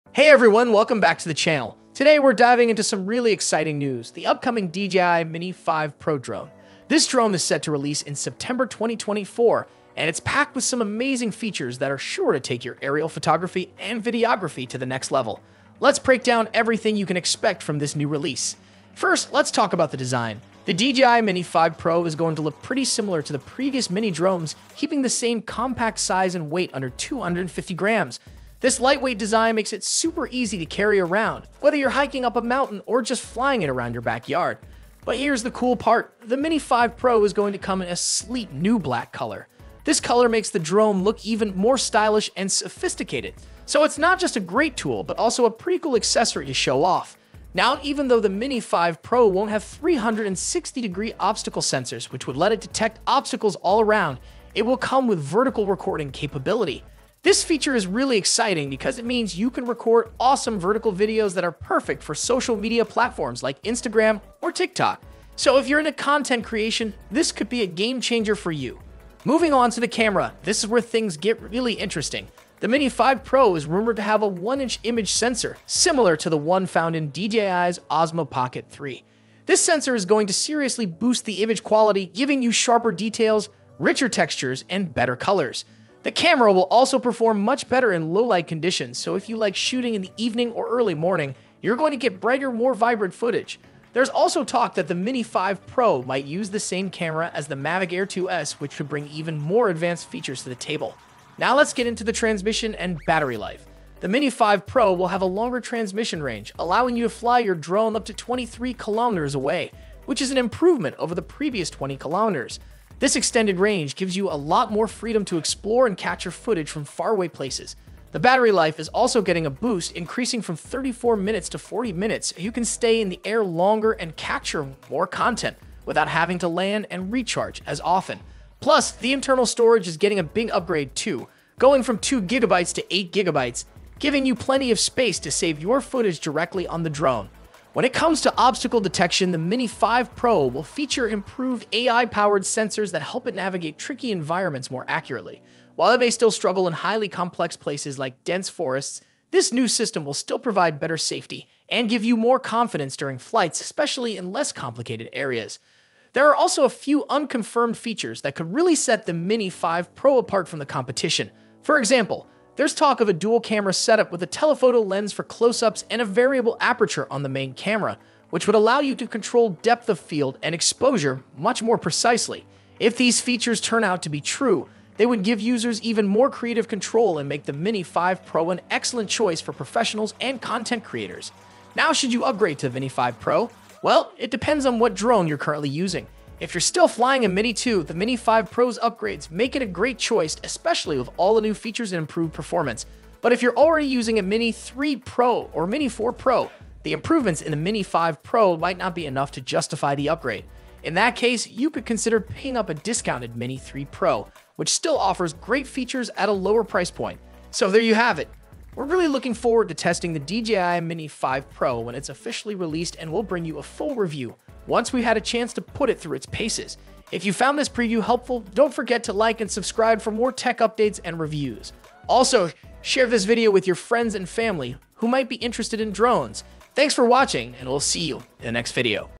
DJI Mini 5 Pro - The Ultimate Guide Features, Release Date, Price, and More
The DJI Mini 5 Pro is a compact drone that is set to be released in September 2024. It has a 1-inch sensor and can shoot 4K videos at 60fps. It has 8GB of built-in storage and can fly for up to 40 minutes on a single charge. It is expected to retail for between $750 and $800.

Here are some of the key features of the DJI Mini 5 Pro:

* Improved camera system with a 1-inch sensor
* 4K video recording at 60fps
* Enhanced obstacle avoidance technology
* Increased storage capacity (at least 8GB)
* Longer flight time (up to 40 minutes)
* New battery and charging technologies

The DJI Mini 5 Pro is expected to be a versatile and valuable tool for both aerial photography and videography. It will be available for purchase in September 2024.